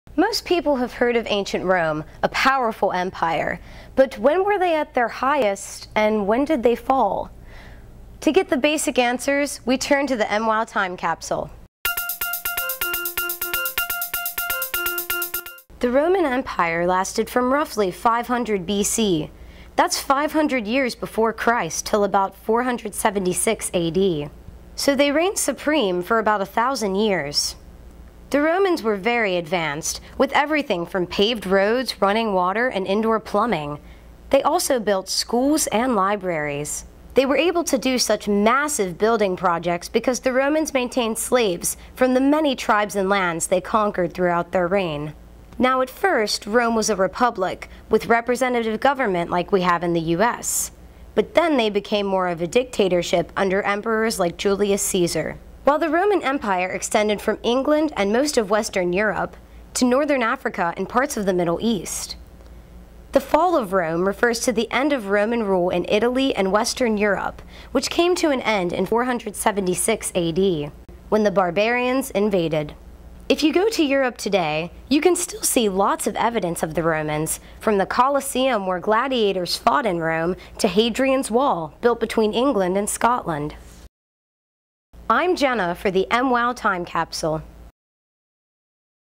roman capsule.avi
As part of the MWOW Network's comparison of the Betts House to Ancient Rome, producers developed a lightning-fast primer on Rome for students - all in less than 2 minutes!